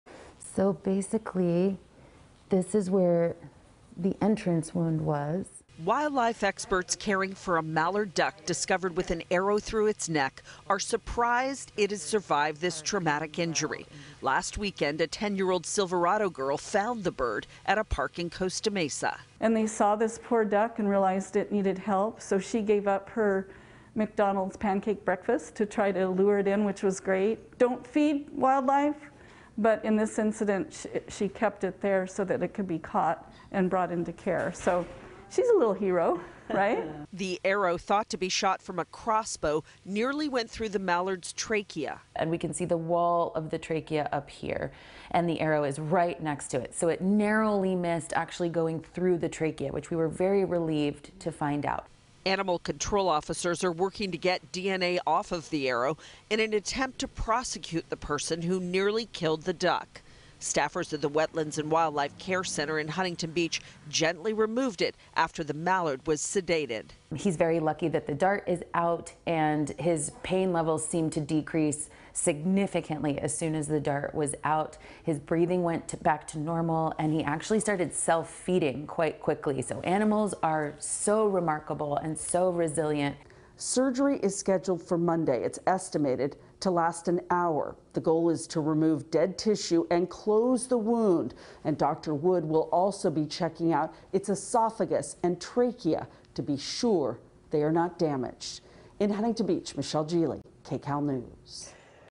Orange County girl hailed as hero after saving duck shot through head with arrow
An Orange County teenager is being hailed as a hero after she took matters into her own hands upon finding a duck with an arrow through its head. Now, while the duck receives veterinary care, authorities are searching for the suspect who committed the obscene animal cruelty. Michele Gile reports.